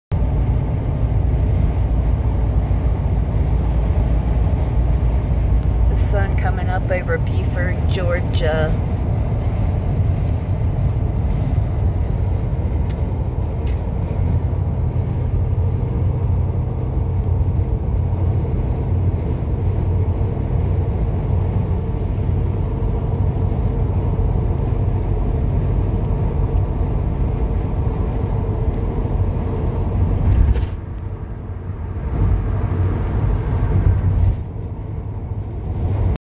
The sun rising as we leave Atlanta and head into South Carolina. van life 2022
it was beautiful.